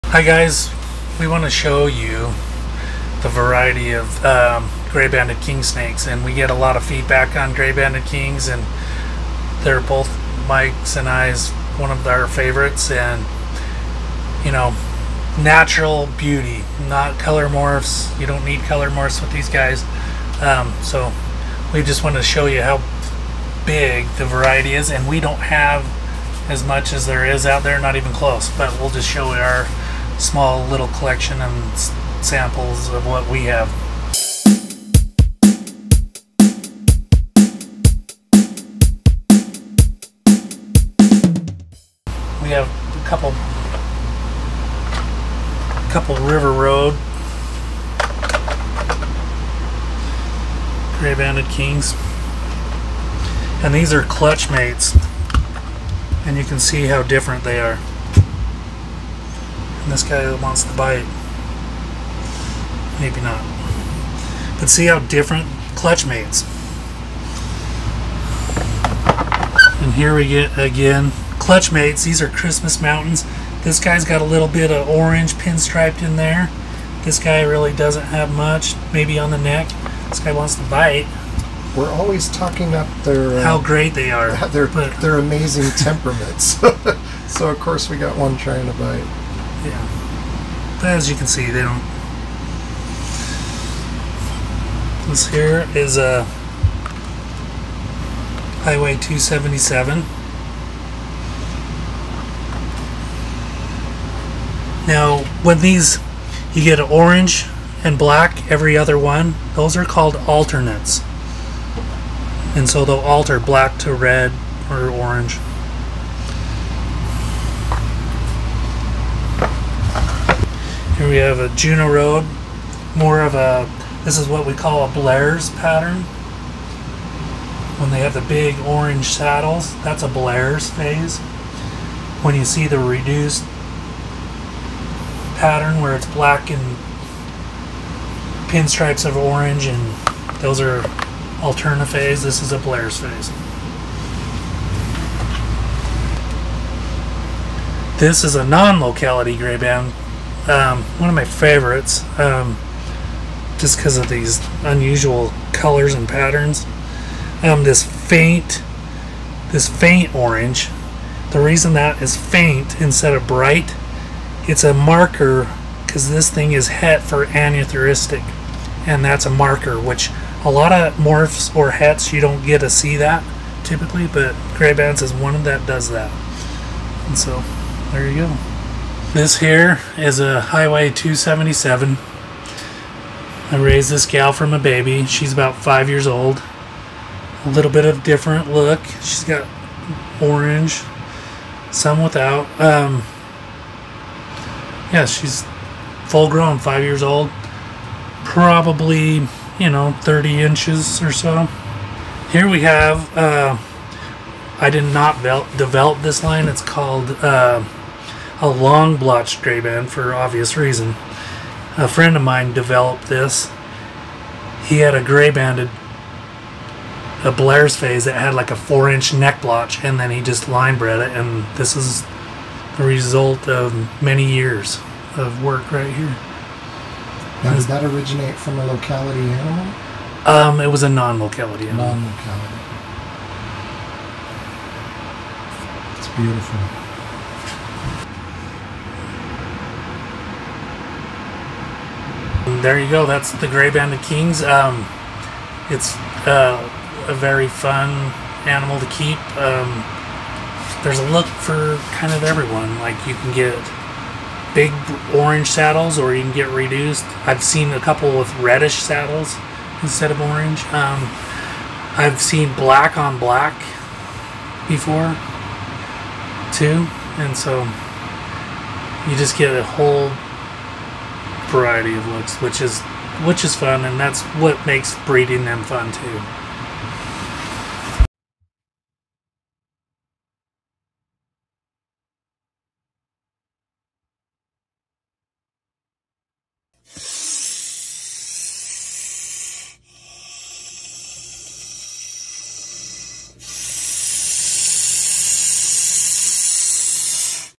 GRAY-BANDED KINGSNAKE variability. Something for everyone.
Jason Nelson, Envy Reptiles, shows some of the varied looks of gray-banded kings. Whether you prefer bright colors or grays and blacks, this species has something for everyone. Not to mention great temperaments and smaller size than most kings.